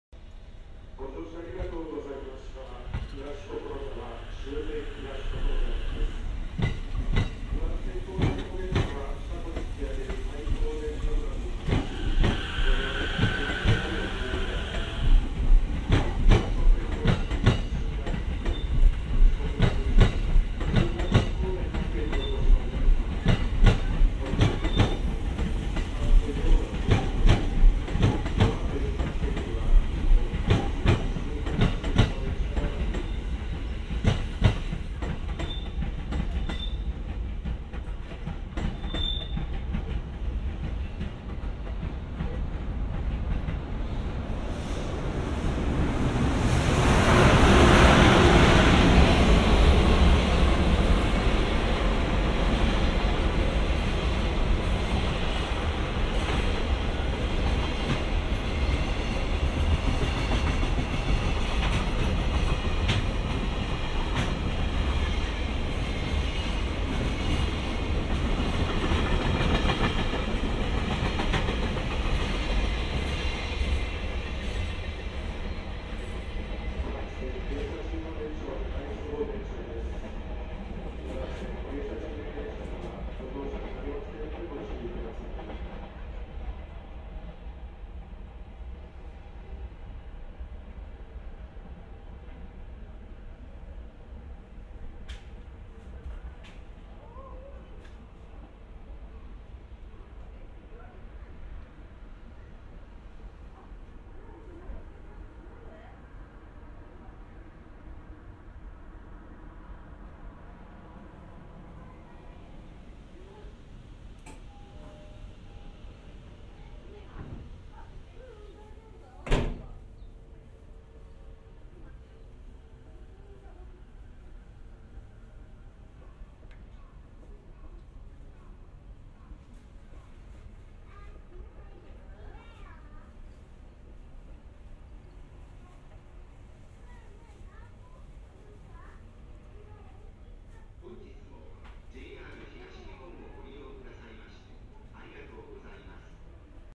2018/12/07 【入区】 武蔵野線 E231系 MU31編成 東所沢駅
JR東日本
武蔵野線
普通 879E
府中本町 ～ 東所沢
E231系 MU31編成
東所沢駅
2018年12月7日